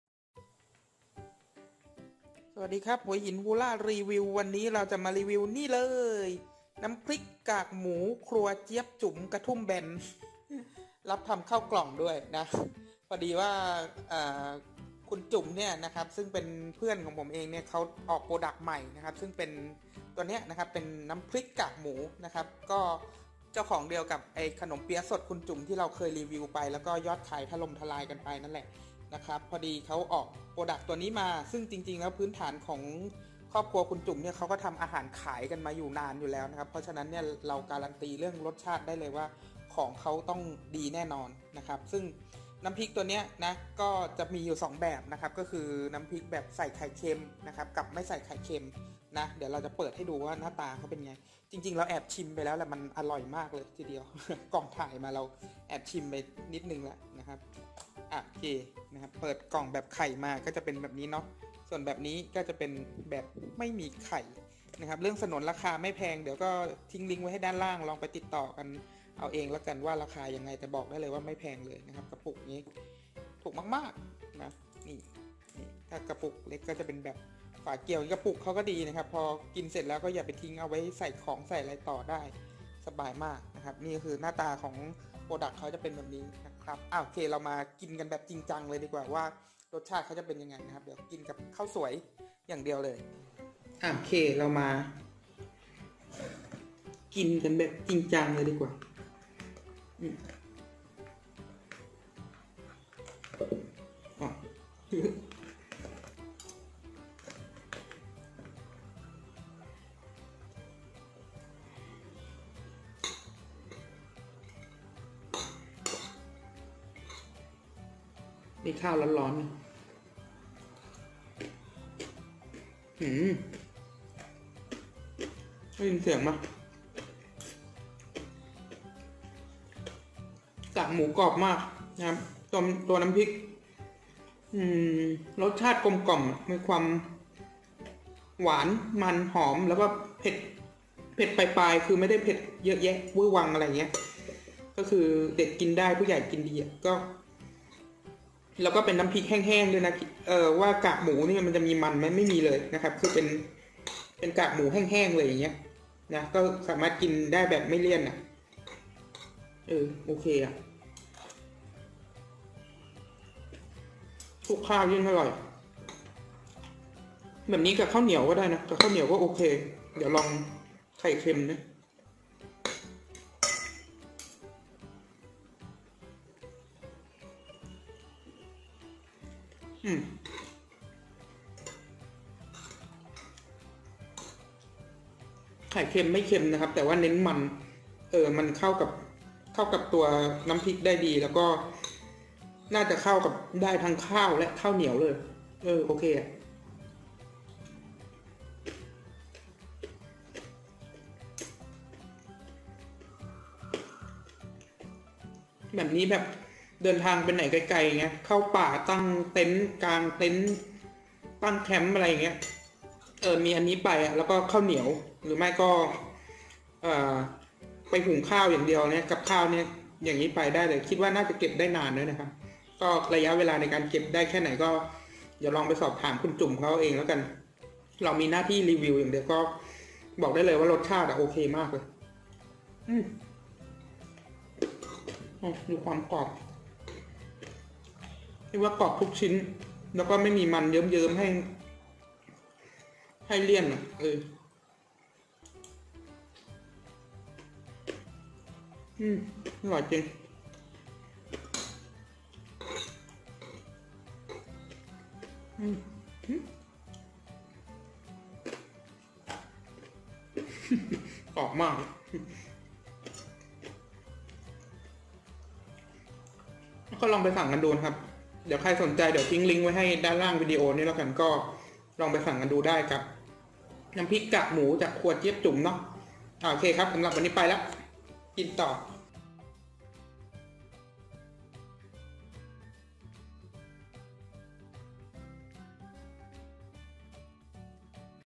รีวิว น้ำพริกกากหมู ครัวเจี๊ยบจุ๋ม กระทุ่มแบน รสกลมกล่อม กรอบทุกชิ้น กินเพลิน ไม่เลี่ยน
ของเค้ากรอบจริงอร่อยจัง